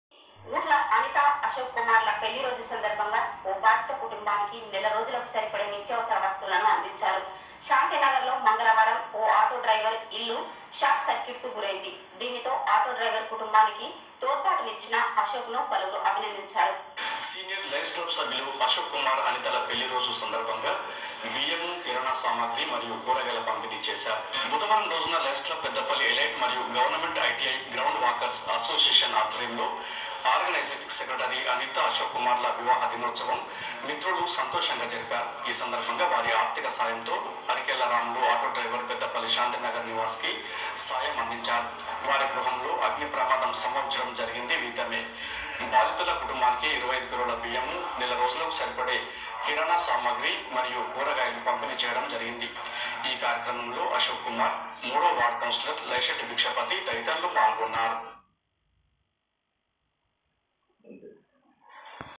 అనిత అశోక్ కుమార్ గార్ల వివాహ దినోత్సవ సందర్భంగా బియ్యము* *కిరాణా* *సామాగ్రి* *కూరగాయల* *పంపిణీ
తేదీ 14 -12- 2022వ రోజున లైన్స్ క్లబ్ పెద్దపెల్లి ఎలైట్ మరియు గవర్నమెంట్ ఐటిఐ గ్రౌండ్ వాకర్స్ అసోసియేషన్ ఆధ్వర్యంలో ఆర్గనైజింగ్ సెక్రటరీ అనిత అశోక్ కుమార్ గార్ల వివాహ దినోత్సవ సందర్భంగా వారి ఆర్థిక సాయంతో అరికెళ్ల రాములు ఆటో డ్రైవర్ పెద్దపల్లి శాంతినగర్ నివాసి  వారి గృహంలో అగ్ని ప్రమాదం సంభవించడం జరిగింది వారికి 25 kgs బియ్యము, నెల రోజులకు సరిపడే కిరాణా సామాగ్రి మరియు కూరగాయలు పంపిణీ చేయడం జరిగింది. ఈ కార్యక్రమంలో  అశోక్ కుమార్,   మూడో వార్డ్ కౌన్సిలర్ లైసెట్టి బిక్షపతి  ఇతరులు పాల్గొన్నారు.